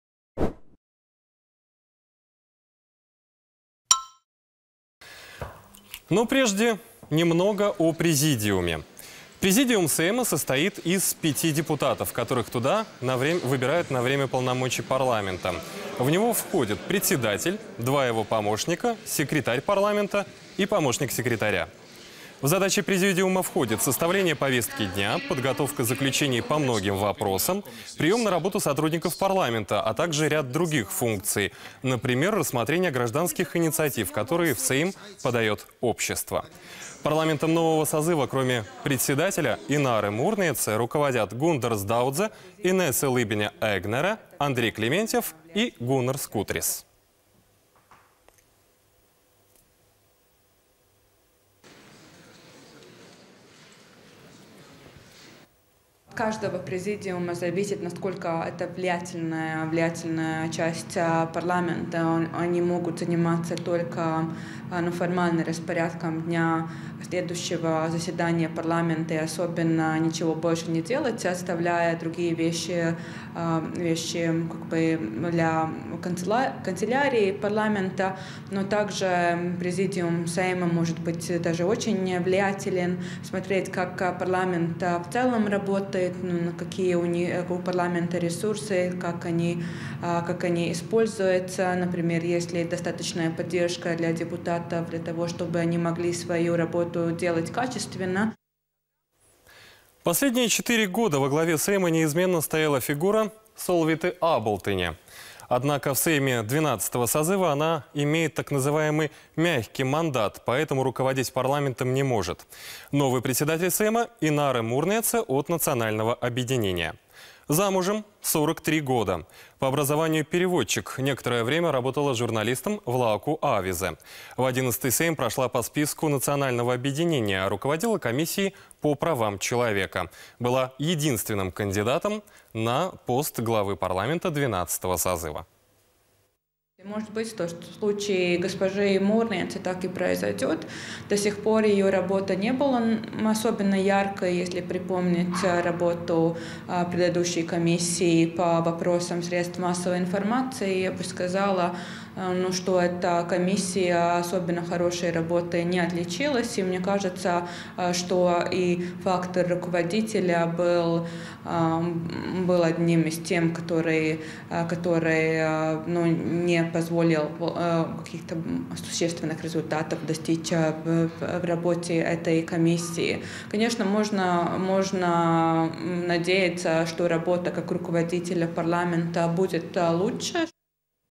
Кто вы, госпожа спикер?
На протяжении последних двух недель мы знакомили вас с министрами нового правительства. Сегодня — внимание уделим парламенту — его председателю и президиуму.